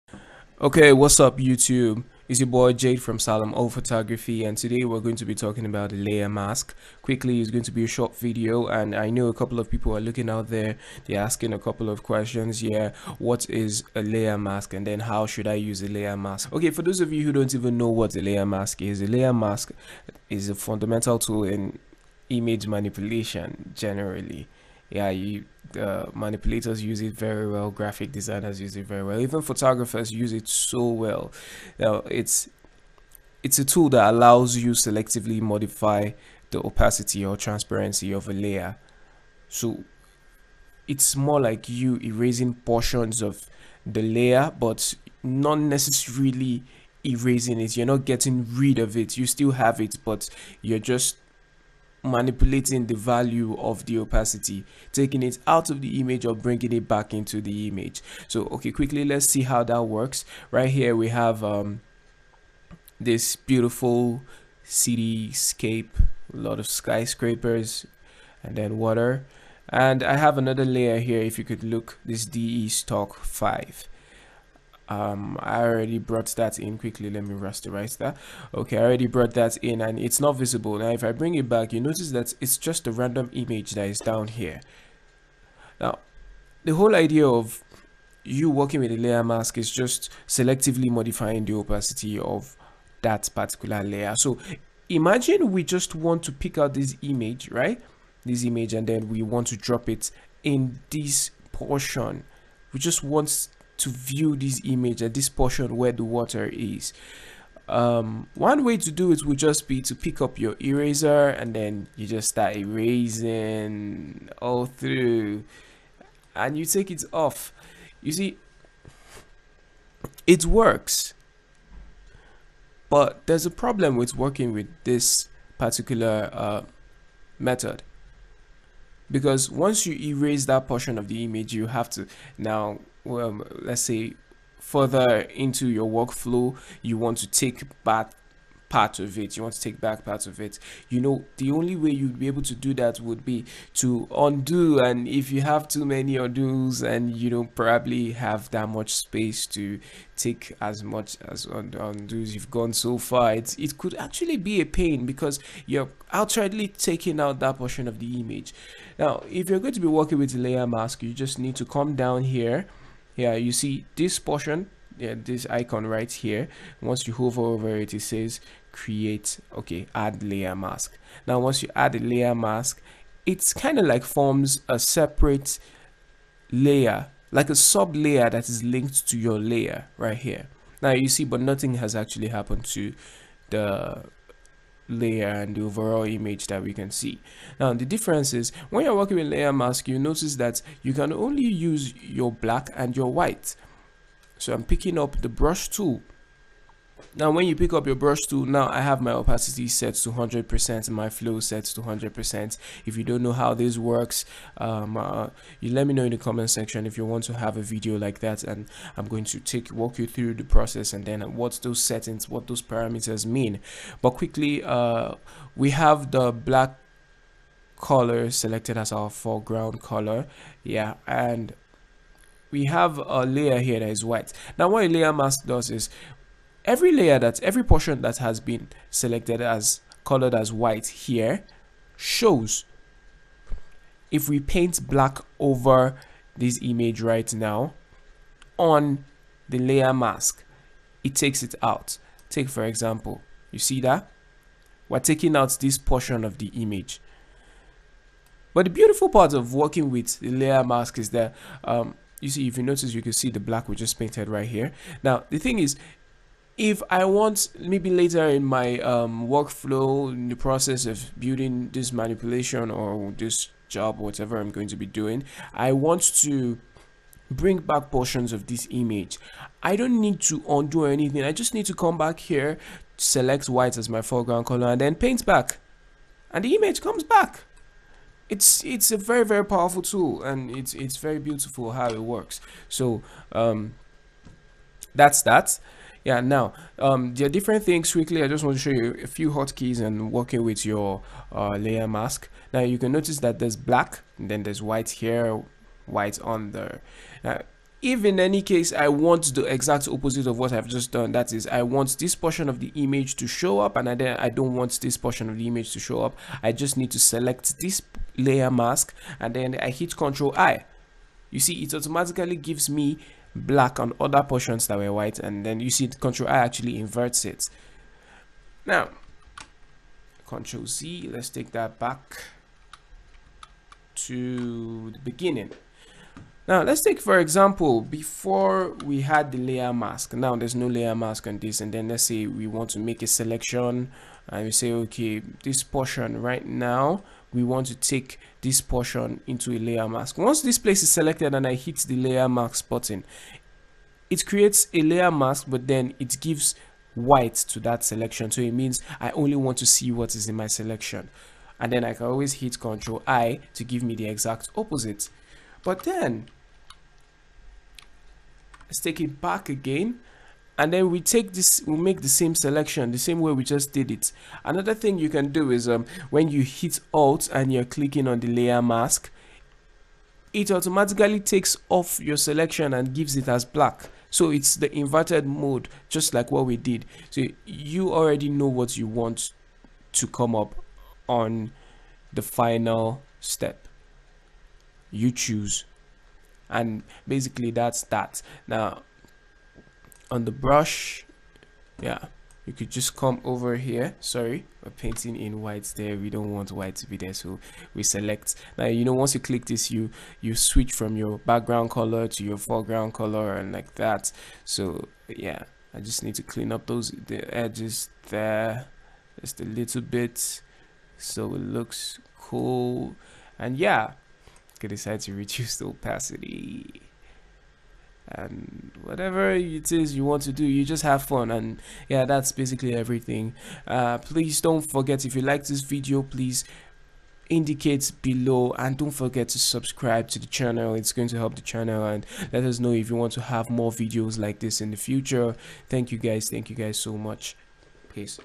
Layer Masking - Understanding the Layer Mask tool
A brief overview to understanding how to use the layer mask function in Photoshop.